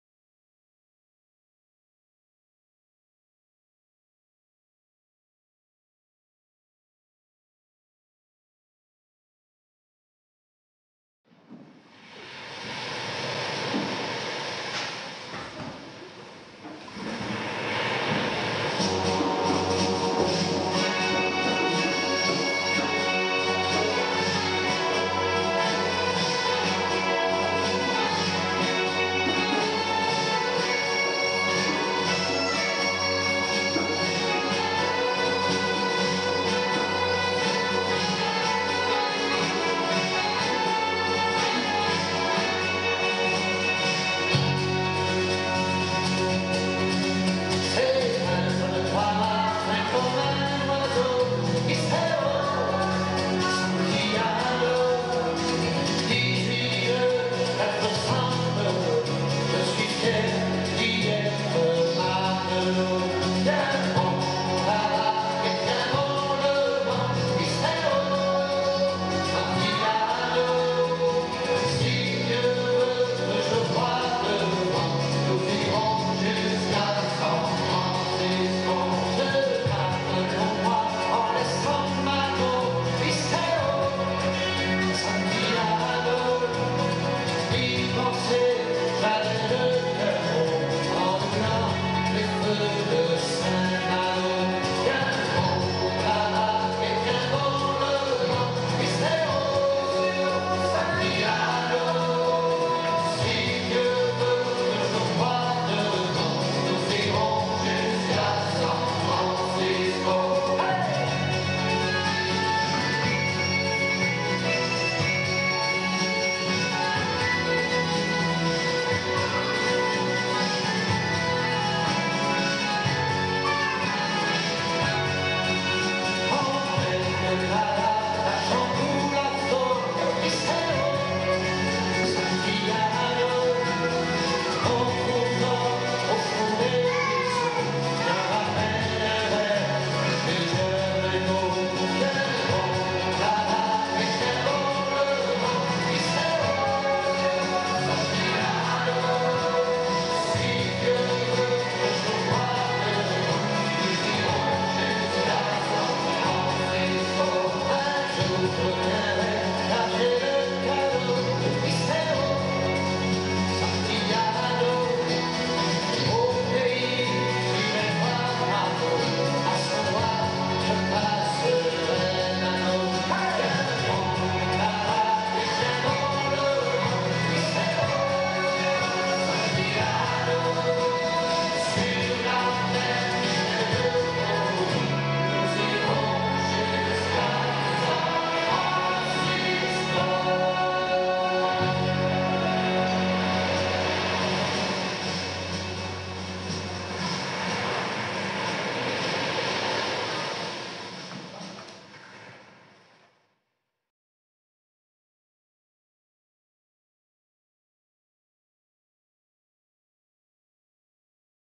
SANTIANO alsace
atelier de théâtre en 2008 dans le nord de l'Alsace à erckartswiller